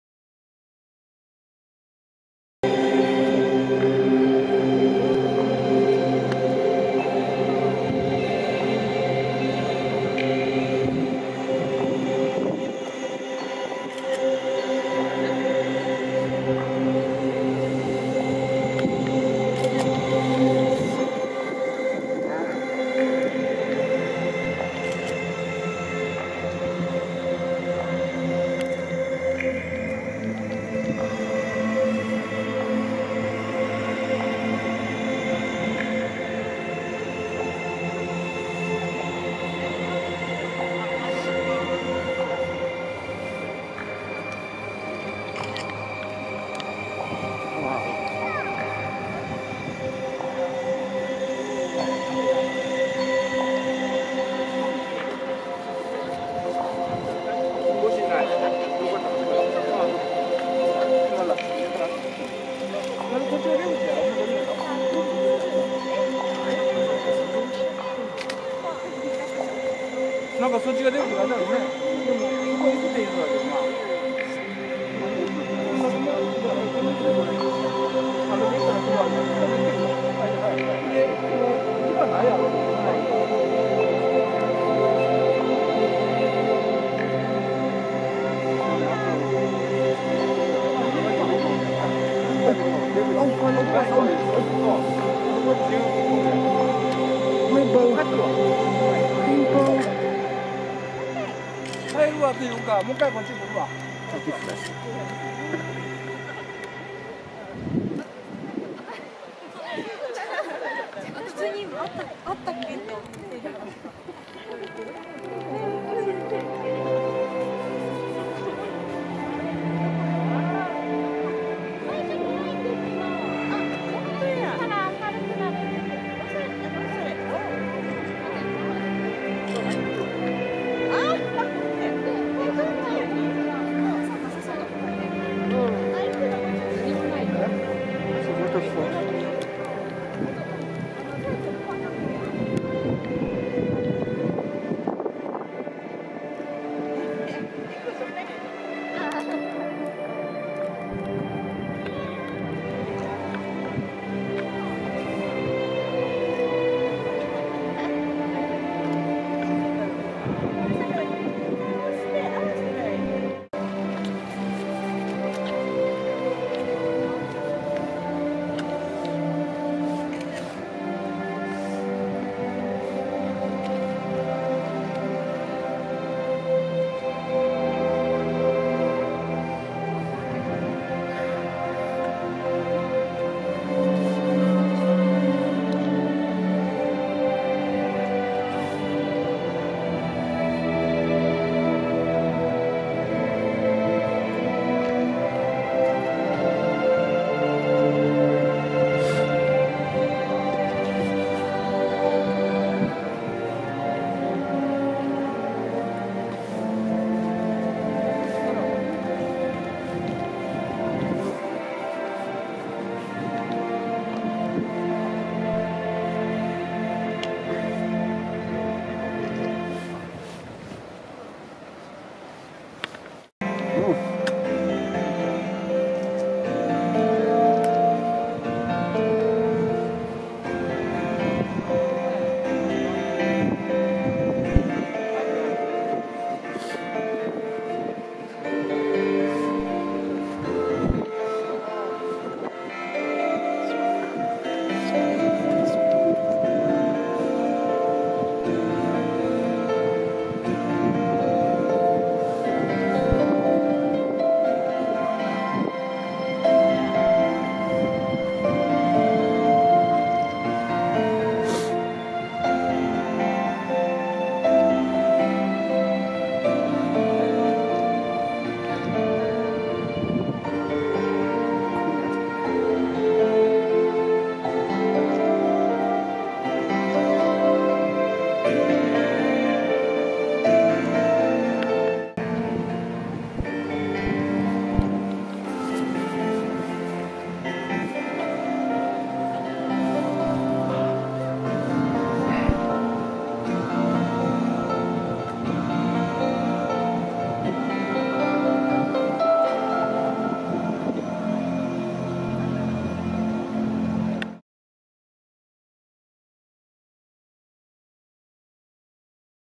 [2019] Nagashima Resort Nabana no Sato "Hikari-no-taiga" Illumination なばなの里「光の大河」
City: Nagashima, Japan
Location: Nagashima Resort
Camera: Panasonic Lumix TZ90/91